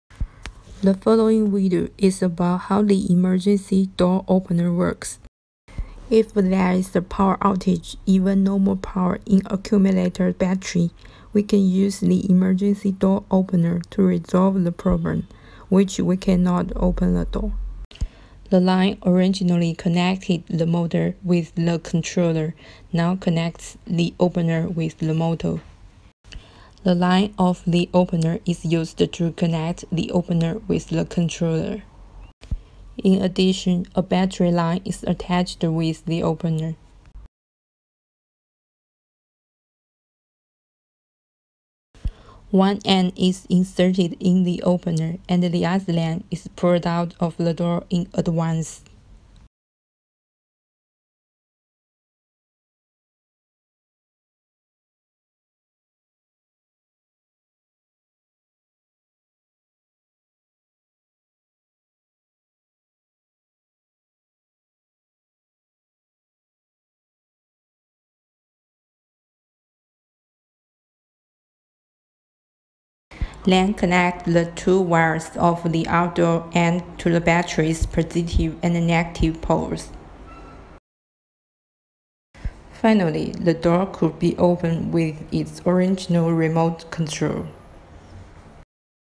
Emergency door opener fo ANLIN dc motors when there is no mains electricity
The following video is about how the Emergency door opener works：If there is a power outage even no more power in the accumulator battery, we can use the Emergency door opener to resolve the problem which we can not
The line originally connected the motor with the controller now connects the opener with the motor.
The line of the opener is used to connect the opener with the controller.
In addition, a battery line is attached to the opener.
One end is inserted in the opener, and the other end is pulled out of the door in advance.
Then connect the 2 wires of the out-door end to the battery’s positive and negative poles.
Finally，the door could be open with its original remote control.